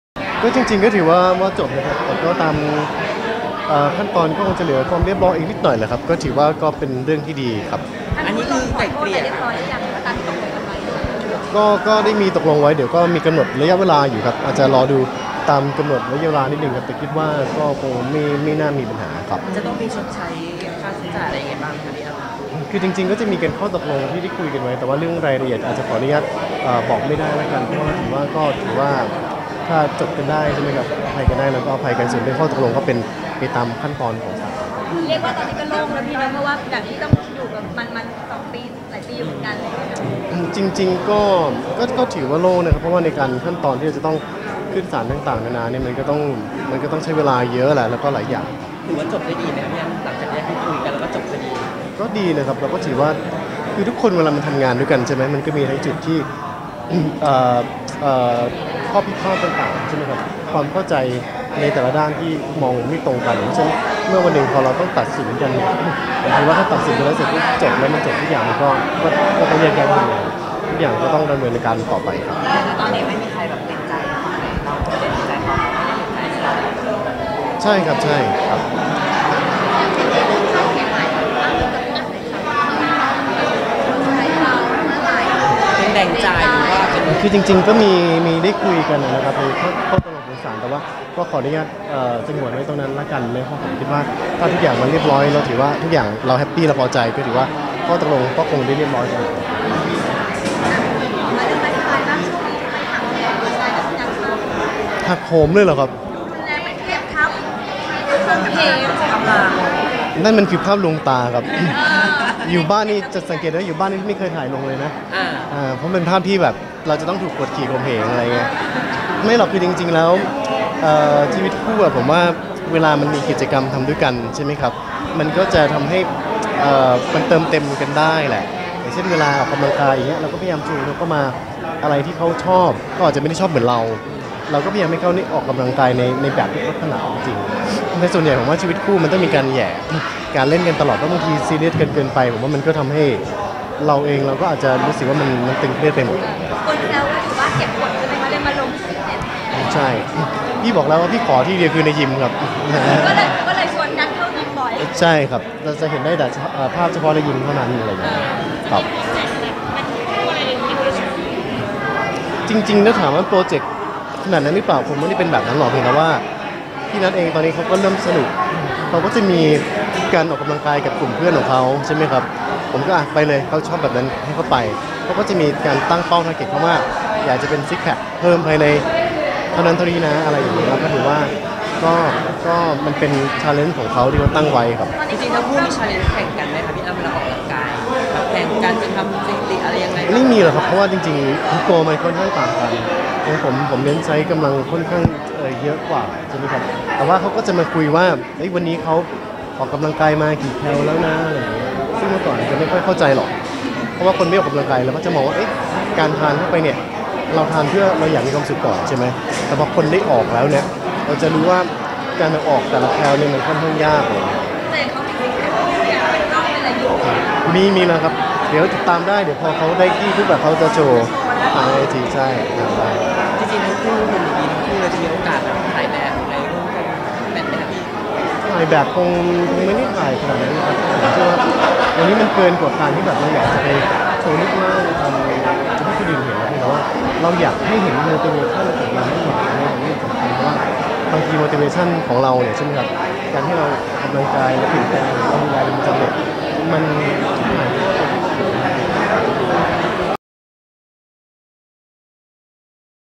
อั้ม อธิชาติ โล่งอก! จบคดีความ รอคู่กรณีชดใช้
อั้ม อธิชาติ โล่งจบเรื่องคดีฟ้องบลูริบบอน ไม่ขอลงละเอียดคดีความและจำนวนเงินที่อีกฝ่ายต้องชดใช้ ให้เป็นไปตามที่ศาลตัดสิน ขำๆชวนนัทเข้าฟิตเนส แกล้งเล่นใช้กำลังกันแรงๆ บอกตนเก่งได้แค่ในฟิตเนส ยังไม่มีโปรเจคคู่